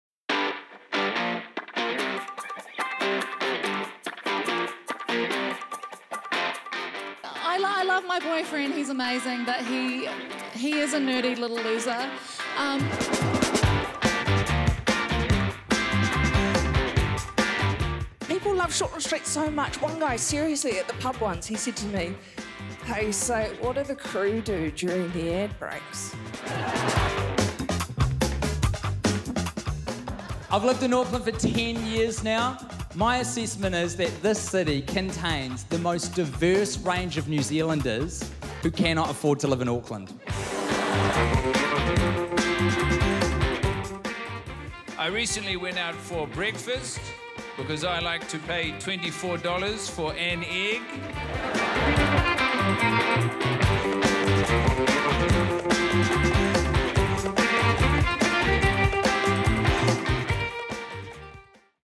2023 Best Foods Comedy Gala Trailer
We have an absolutely stellar line-up of comedians from Aotearoa and around the world joining us for this year's Best Foods Comedy Gala!

Featuring Kura Forrester, Becky Lucas, Tim Batt, Two Hearts, Guy Montgomery, James Nokise, Ray O'Leary, Anne Edmonds, Lloyd Langford, Tom Sainsbury, Becky Umbers, and more to be announced. You do not want to miss this - it will be a-mayo-zing!

This year, the Comedy Festival is celebrating 30 years of great laughs so come along to the Best Foods Comedy Gala and celebrate with us!

Fri 5 May at The Civic, Auckland
Sat 6 May at Michael Fowler Centre, Wellington

2023 NZ International Comedy Festival with Best Foods Mayo
5 – 28 May
comedyfestival.co.nz